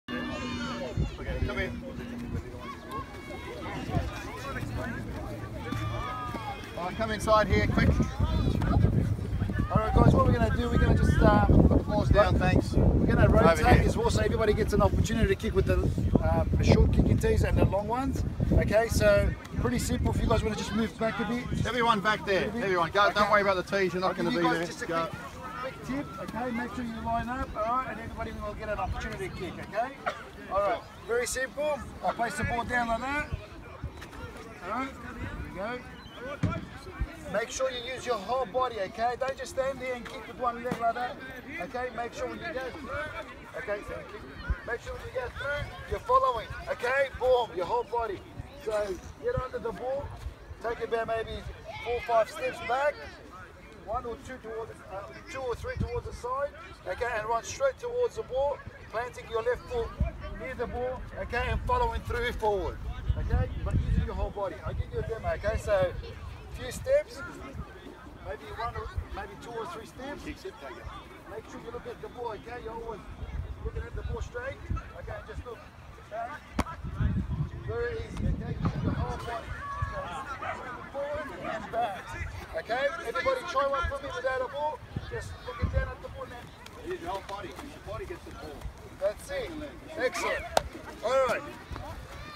Masterclass with Hazem El Masri
Hazem showing the kids proper technique Neptune park 14/07/16